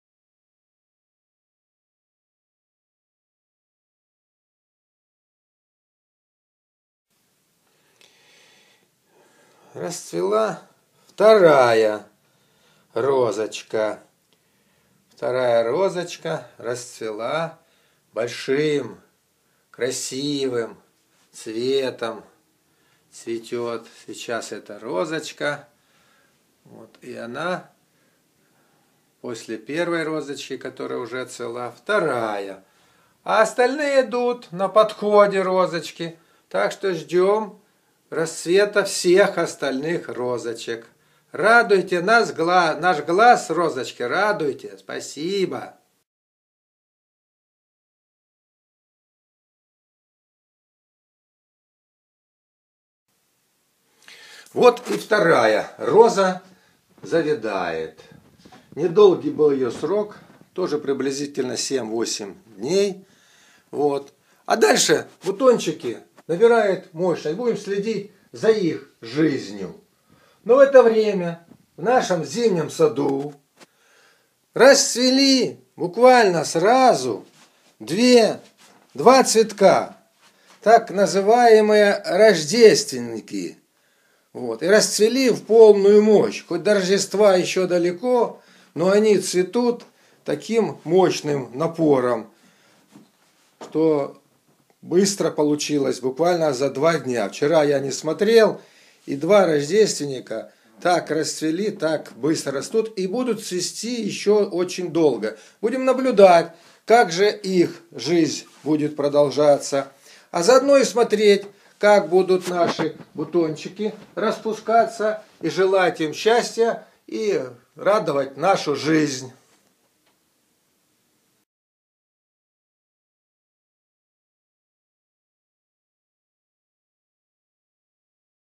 Flower. Home rose. Part 2. / Цветы. Домашняя роза. Часть 2.
Our winter garden. Second blossomed rose.They began to bloom other colors.
Наш зимний сад.Расцвела вторая роза.Начали цвести другие цветы.